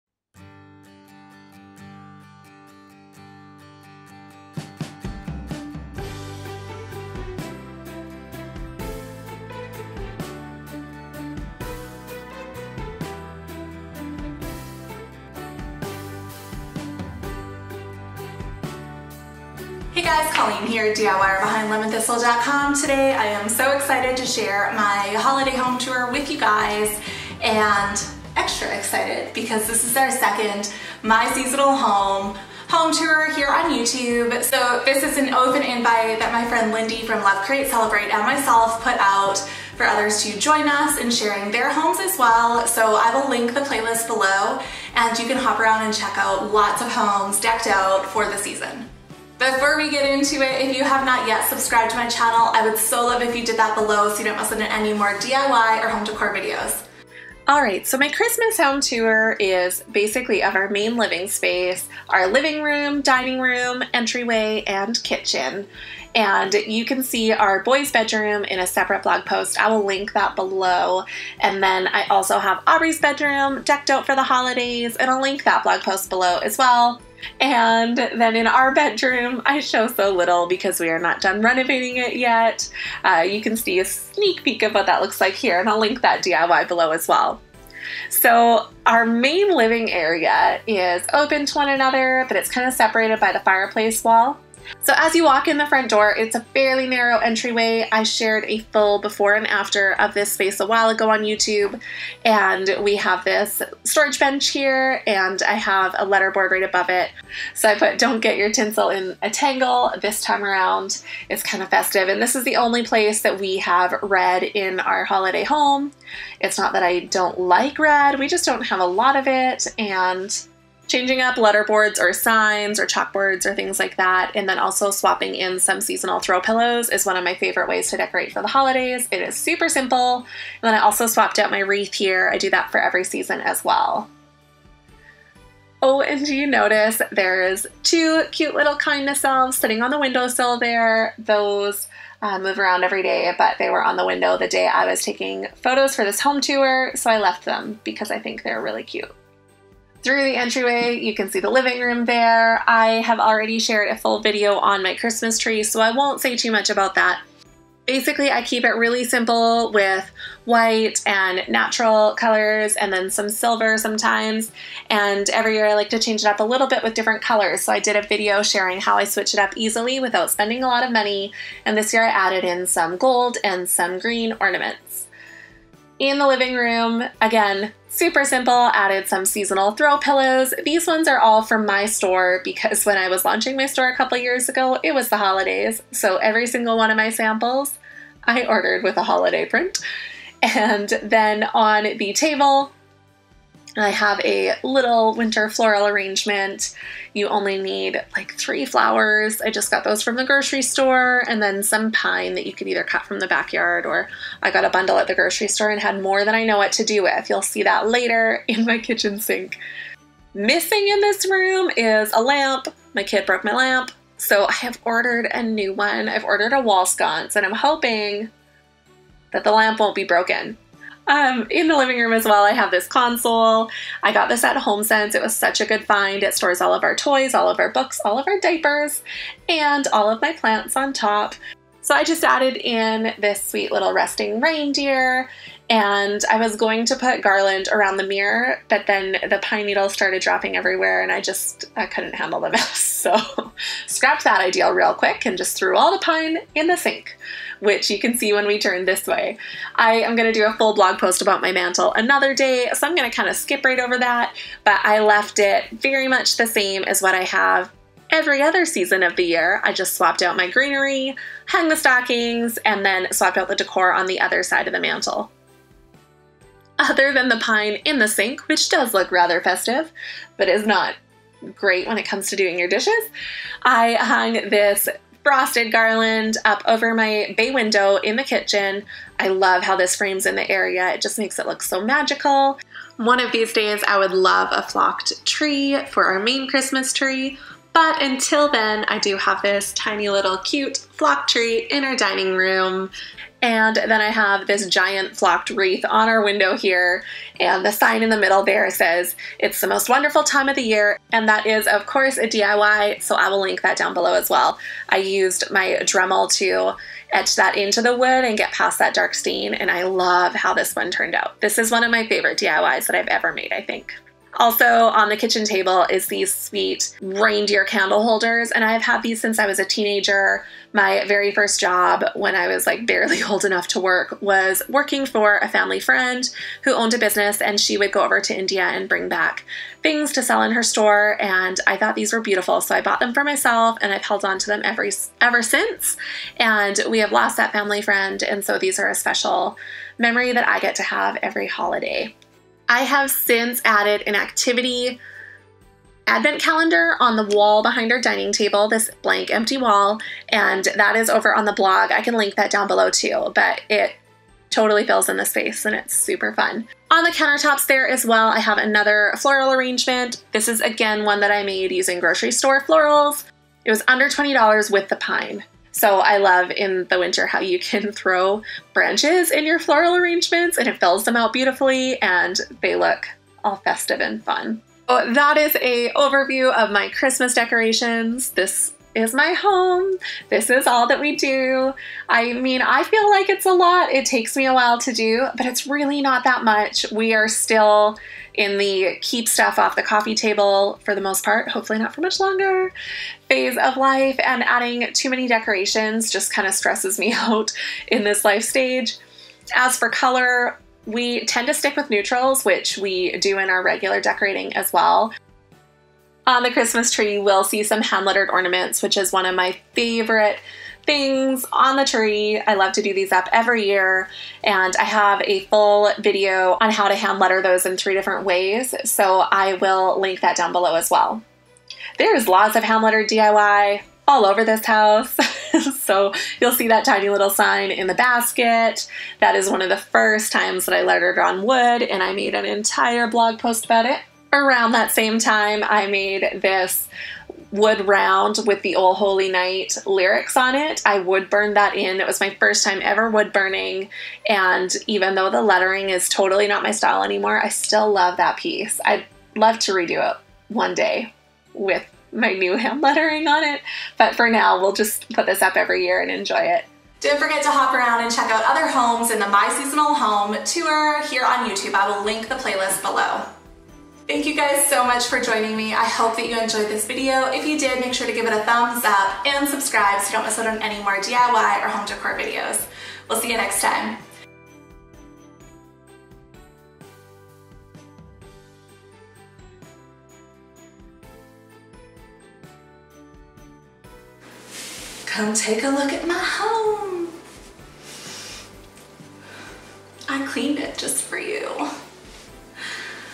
Modern Holiday Home Tour 2019 | My Seasonal Home Holiday Home Tour
Today I'm sharing a tour of my home all decked out for Christmas in this modern holiday home tour 2019! This video is part of an open invite collab I'm co-hosting with my friend Lindi from Love Create Celebrate.

Come take a tour around my house decked for the season- with modern hand lettered decorations and tons of DIYs, we love to decorate with greenery and neutrals- much like the rest of our decor!

My Co-Host Lindi Here: @lovecreatecelebrate

The full photo version of this home tour (pinnable!)